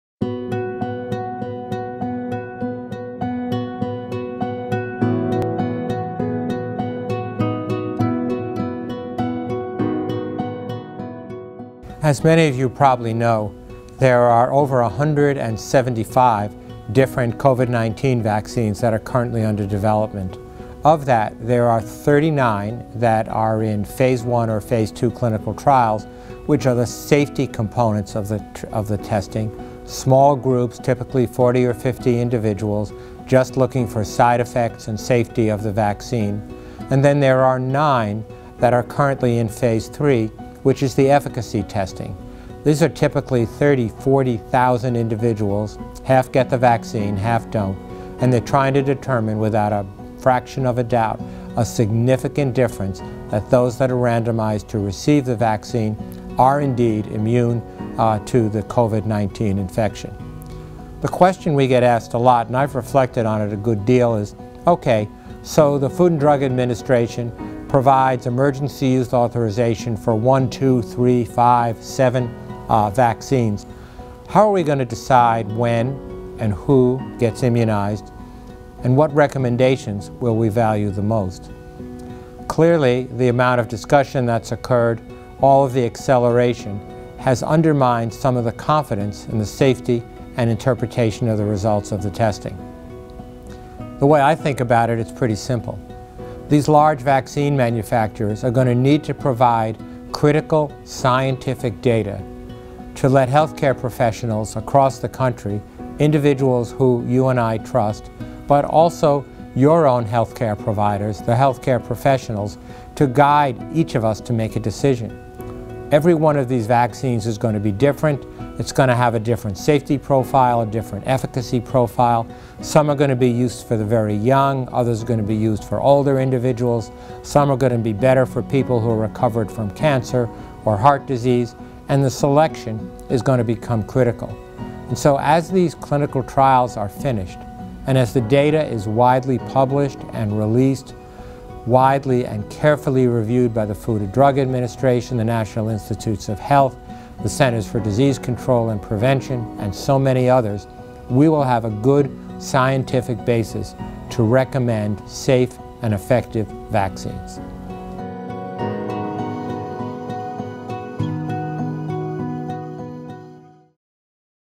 Chancellor Gold. What should I consider when determining whether I get a COVID-19 vaccine?
UNMC and UNO Chancellor Jeffrey P. Gold, M.D., answers your  COVID-19 questions.